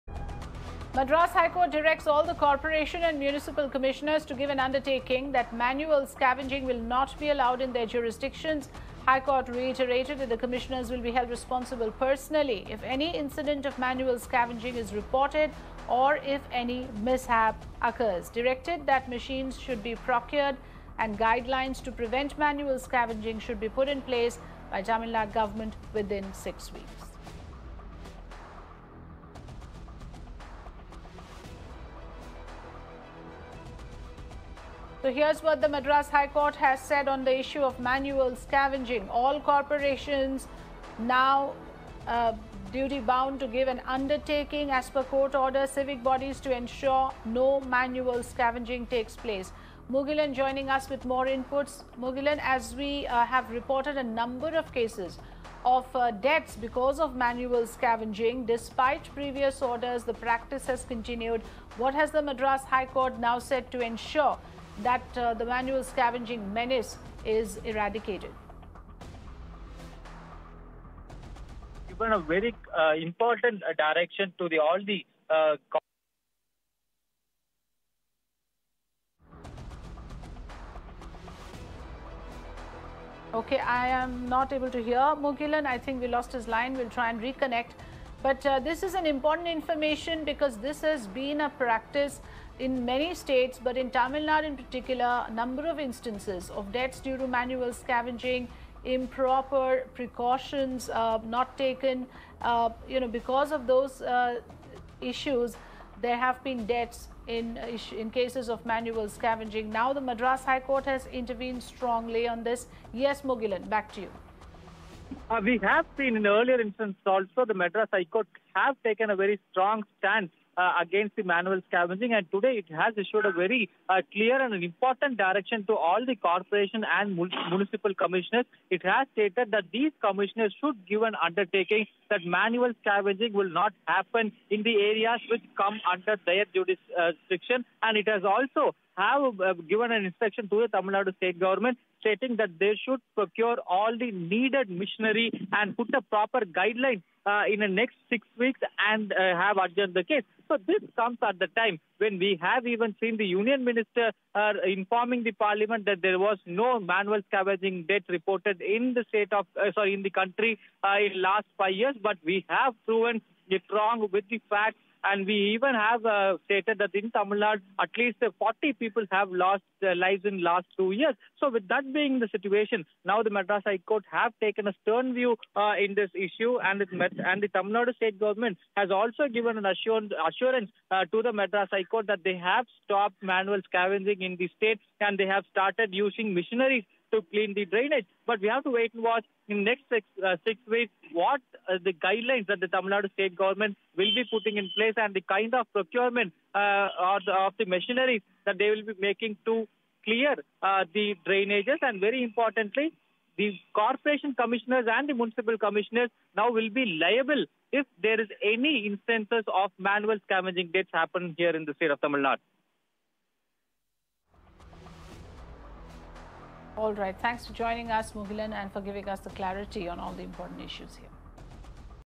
Madras High Court directs Tamil Nadu government over manual scavenging; asks to issue guidelines
The Madras High Court has now issued a notice to the Tamil Nadu government in connection with manual scavenging. The High Court has directed all the corporations to submit an undertaking that will state that there will be no manual scavenging under their jurisdiction. The High Court also observed and reiterated that the commissioners will be held responsible personally if any incident of manual scavenging is reported. The Madras High Court directed that all the machines should be procured and new guidelines are issued within the next 6 weeks. The civic bodies are directed to ensure that no manual scavenging takes place.

Watch now.